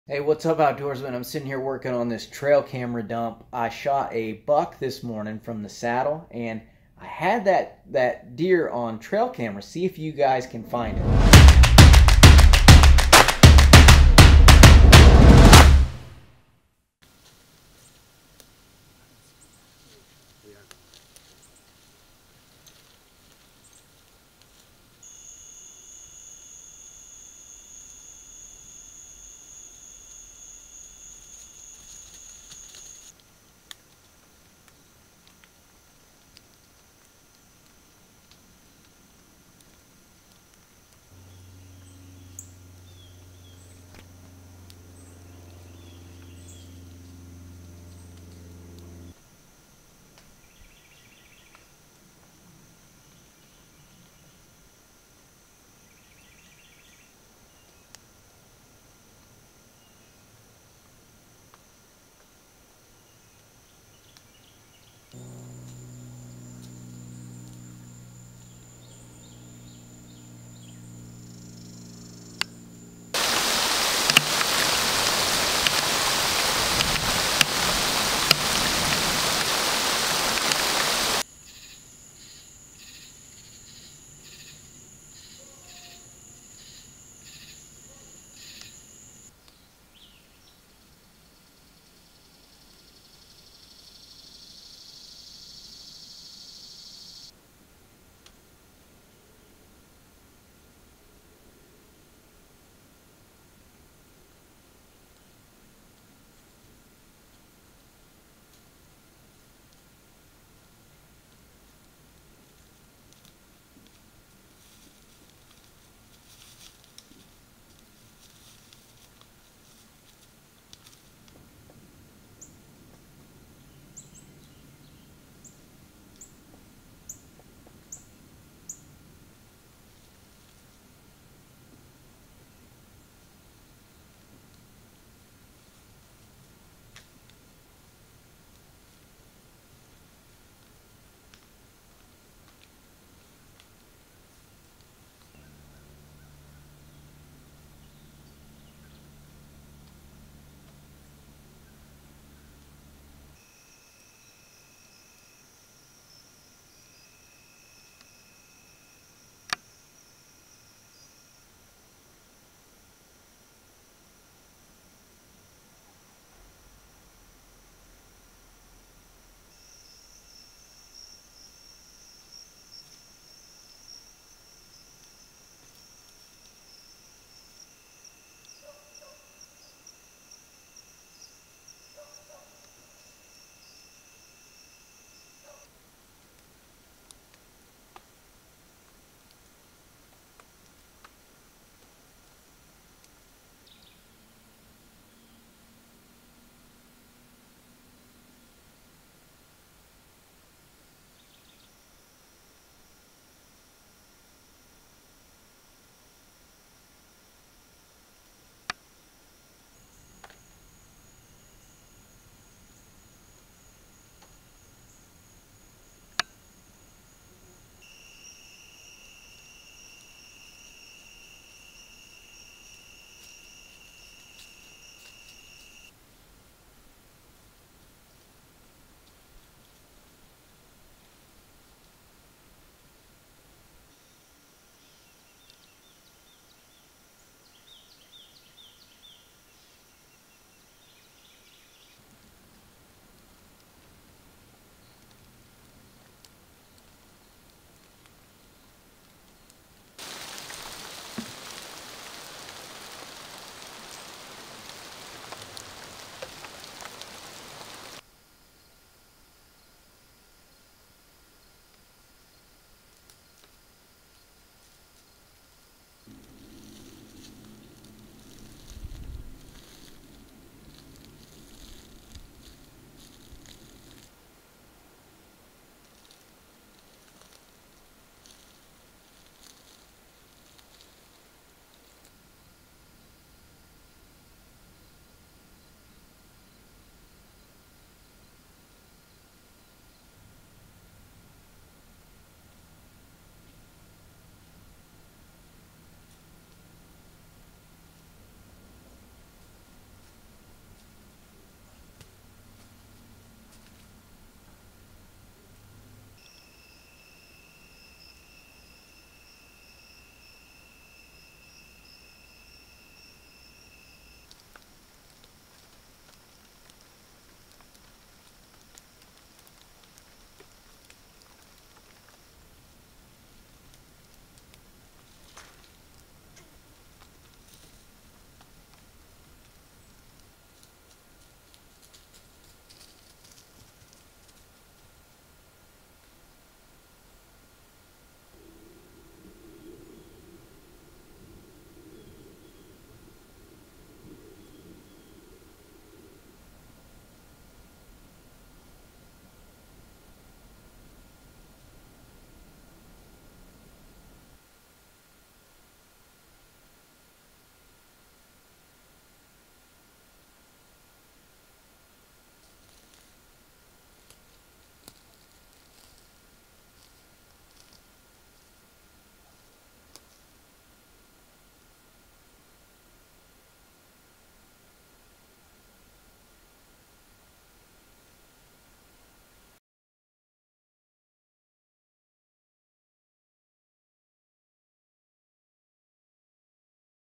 Public Land Trail Camera Dump
This is a 5 month Moultrie Trail Camera dump. The camera was cooking over a trail intersection for 5 months on public land. The last few vides are from the morning I hunted and killed a little 7 pointer.

Want to learn more about saddle hunting? look no further than

AND FINALLY!